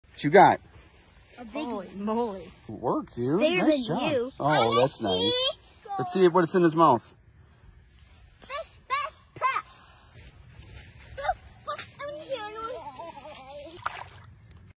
Family Pond Fishing - Berkley Swim Shad
It’s hard to beat a pre rigged @berkleyfishing Swim Shad for the kids when pond fishing, always gets them a bite!